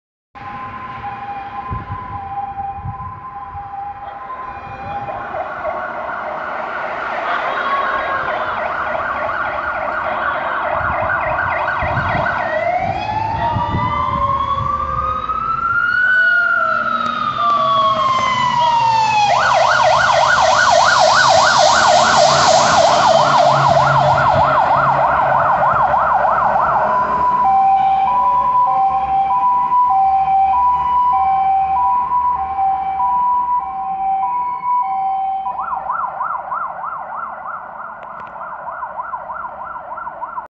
Przejazd alarmowo zastępu GCBA MAN 421[P]25 Z JRG GNIEZNO
Dnia 24 Lutego 2022r.Około godziny 20:55

Przejazd alarmowo zastępu
-GCBA MAN STOLARCZYK 421[P]25 Z JRG GNIEZNO do zakopanej karetki w Trzemesznie